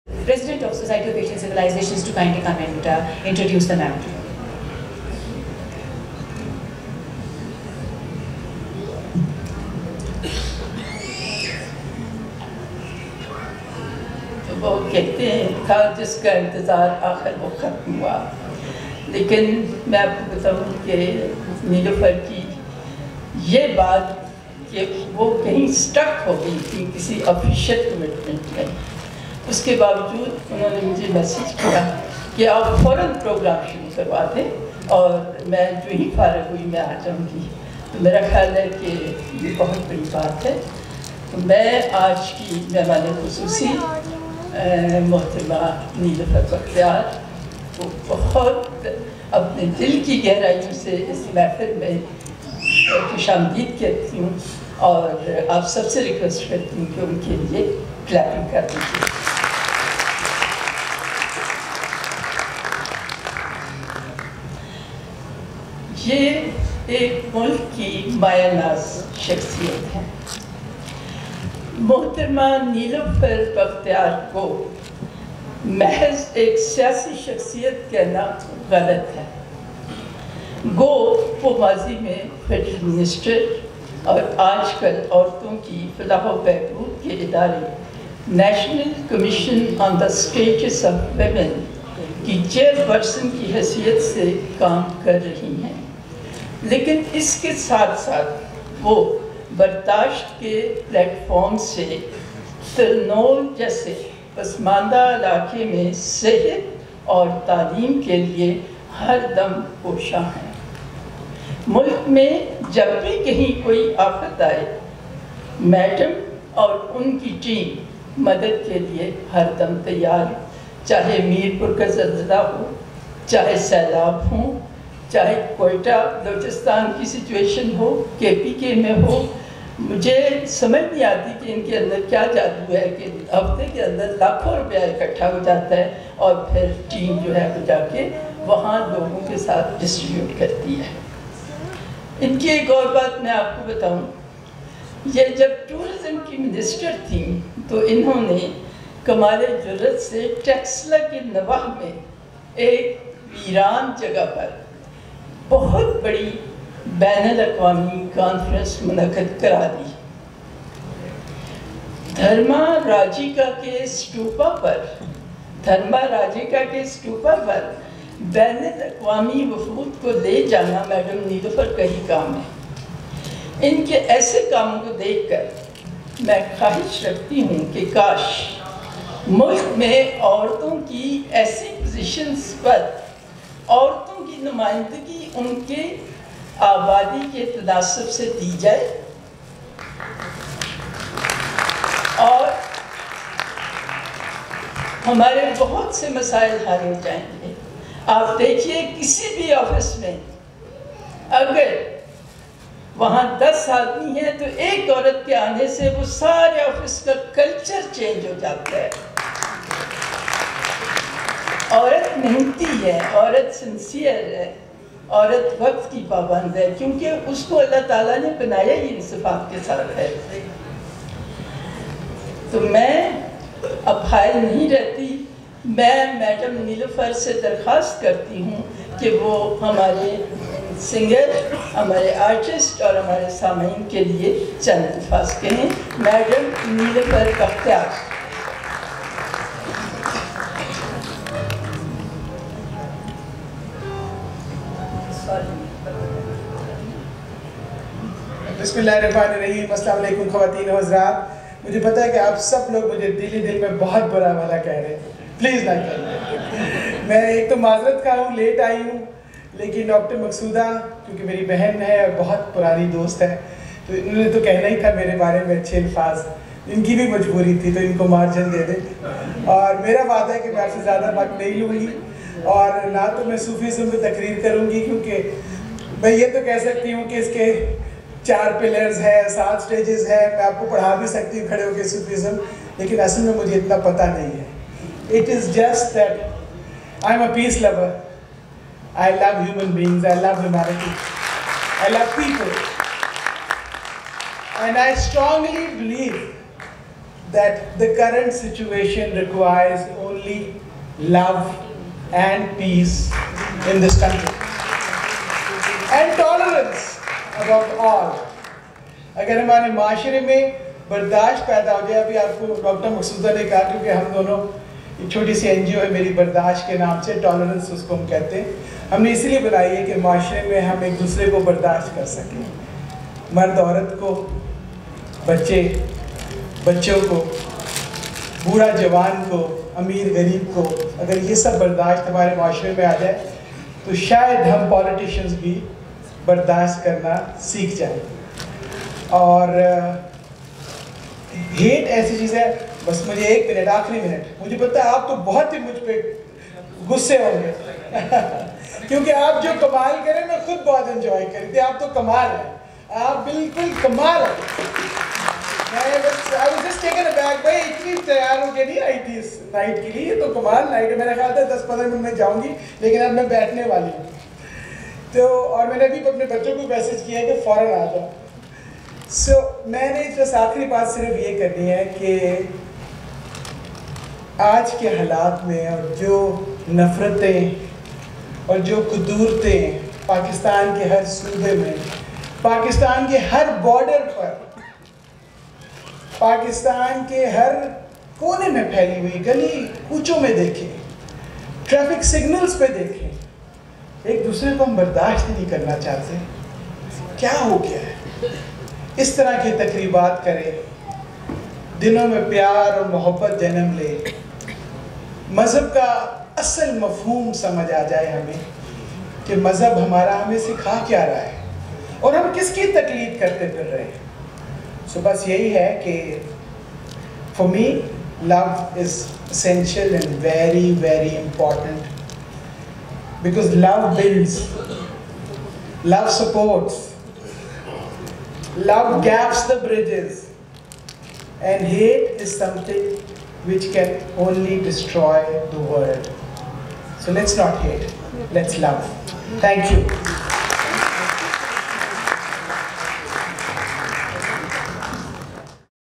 Minister Nilofar Bakhtiar Speech | Qawal Ustad Shabih Sen
An Evening of Sufi Music with Ustaad Shabih Sen at the Pakistan National Council of Arts (PNCA) hosted by Society of Asian Civilizations Pakistan.